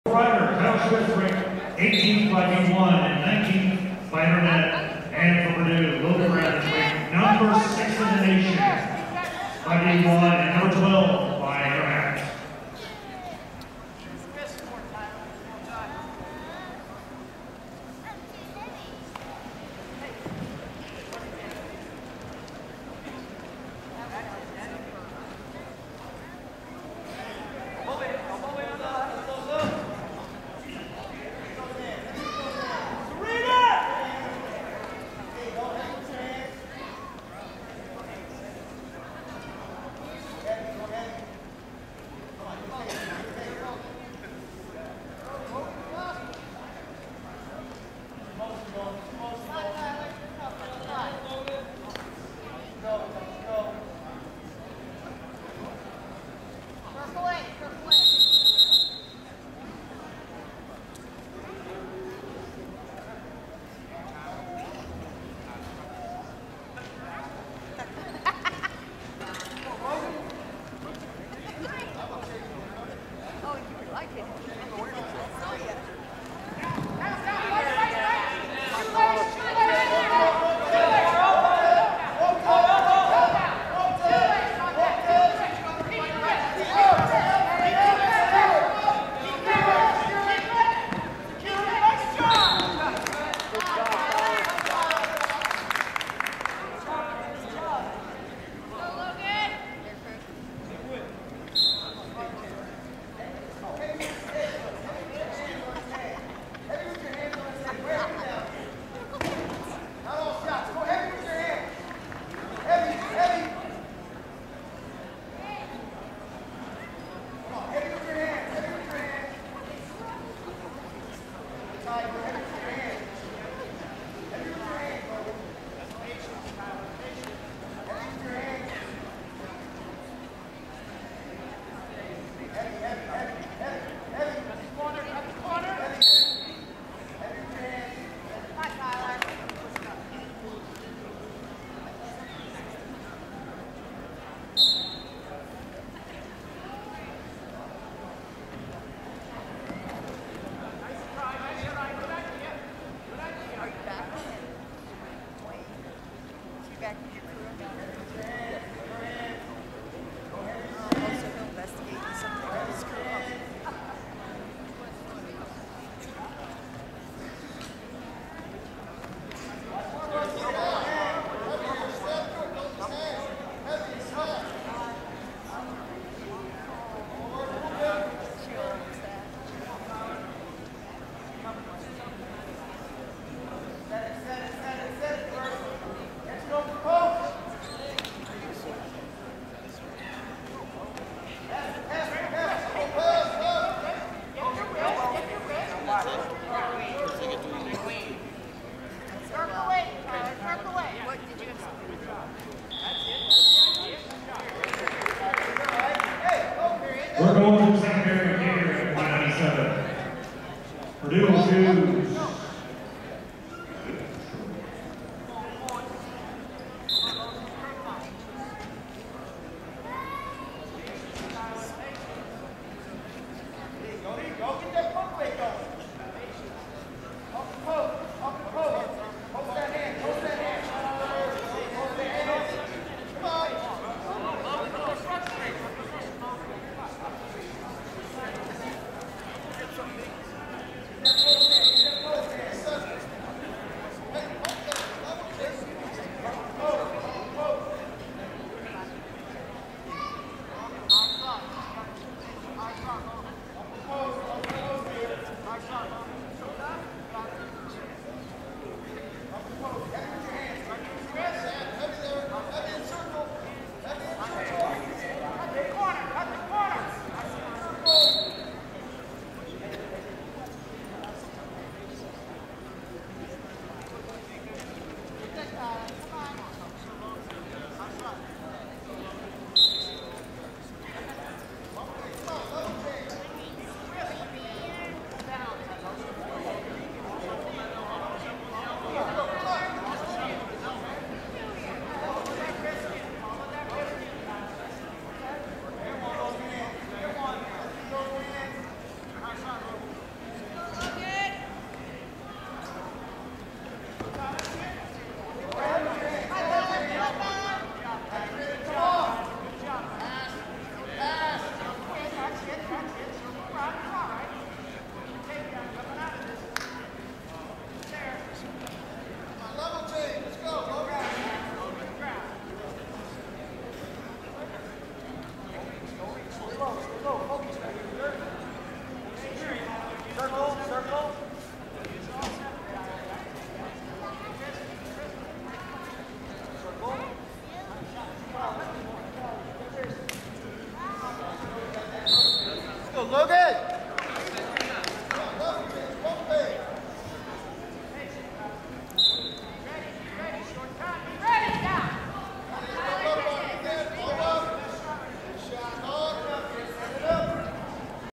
197 LBS: Logan Brown (Purdue) vs. Tyler Smith (Rider)
197 LBS: Logan Brown (Purdue) dec Tyler Smith (Rider), D 3-1. Purdue Wrestling at Old Dominion University, 1/3/2010.